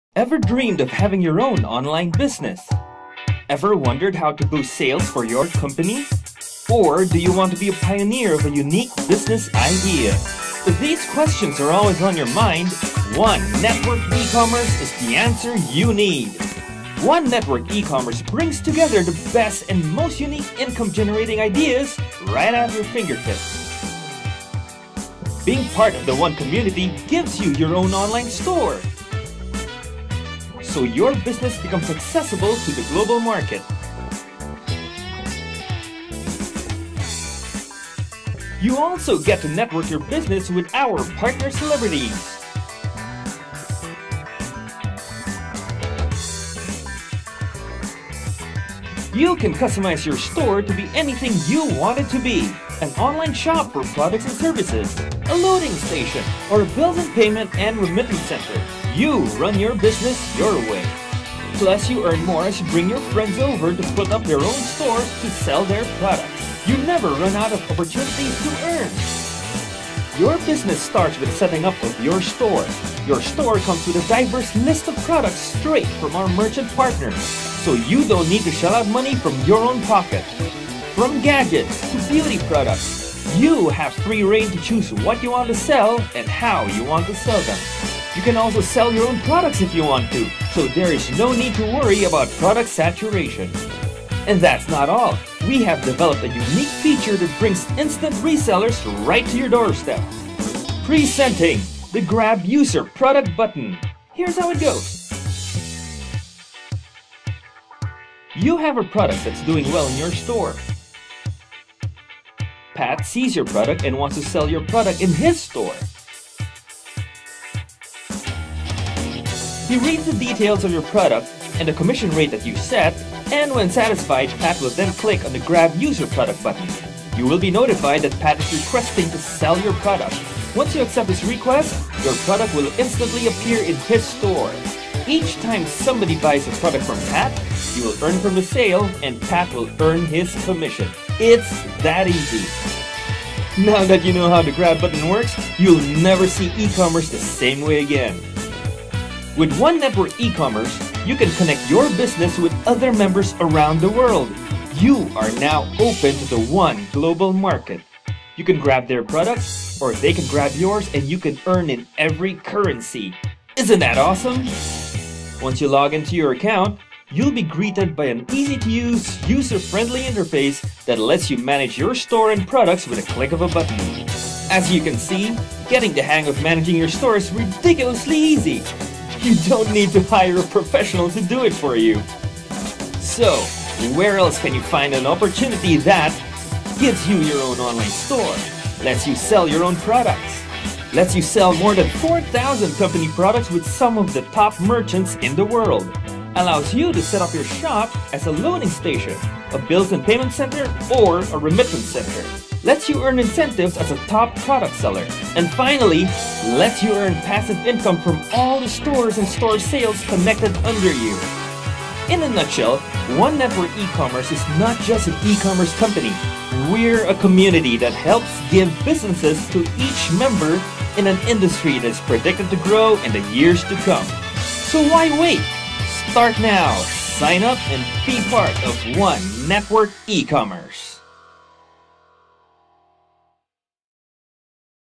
Build your Online Business with ONE
Do you want to work at home and make money online?
Do you want to have your own online store?
Do you want to boost your sales, grow your network and make lots of money?

ONE (One Network eCommerce) is a pioneering eCommerce company helping entrepreneurs create their own online store, sell products, with built in payment facilities and logistics (delivery system).

Good news we did all the hard work for you.

Once inside click "Reserve your Store" button to register and explore the online platform.